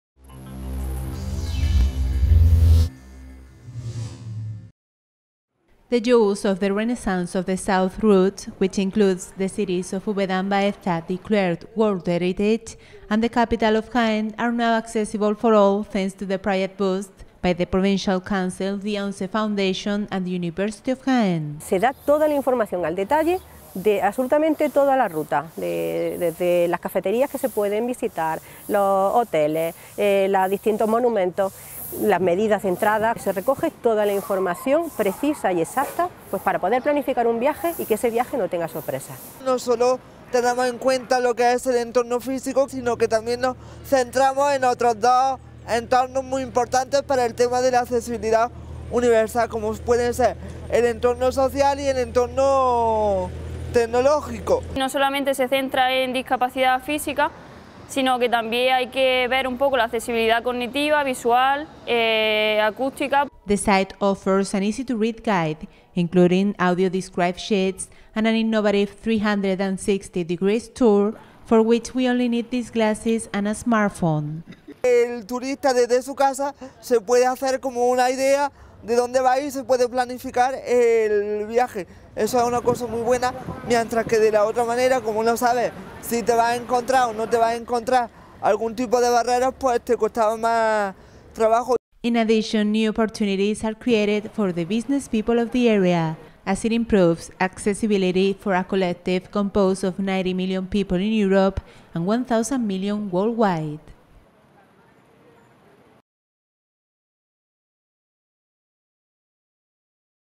AS The Renaissance of the South, inclusive tourism thanks to new technologies and augmented reality
The University of Jaén and the ONCE Foundation have designed an inclusive route called ‘The Renaissance of the South’, which allows universal access to the Andalusian Renaissance jewels, included the World Heritage cities Úbeda and Baeza. This route is part of the promotional actions carried out by the Provincial Council. To design it, they have taken all the existing kinds of functional disabilities into account, and they have used new technologies and augmented really to allow access to inaccessible places.